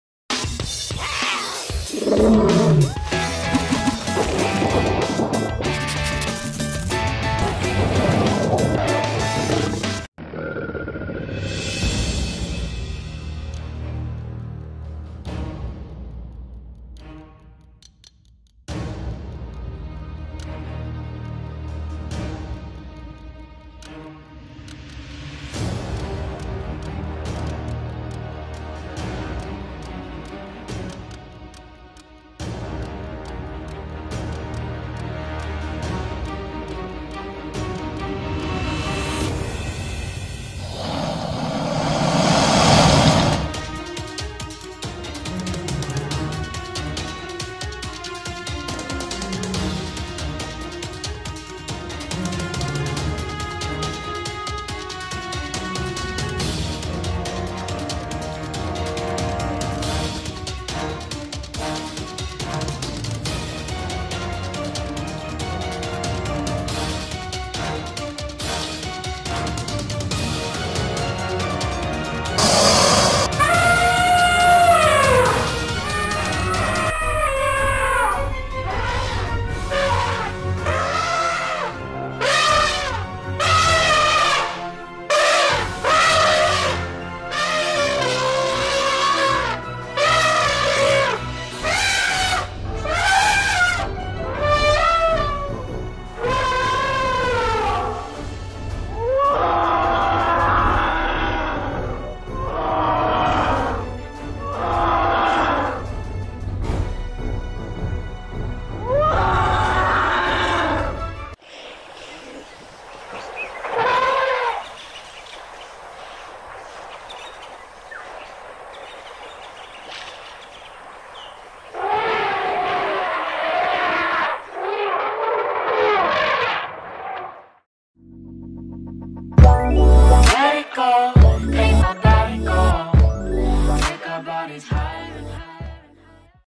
Amazing Elephant Save Baby Elephant
Amazing Elephant Save Baby Elephant From Crocodile Hunting   Animals Hunting Fail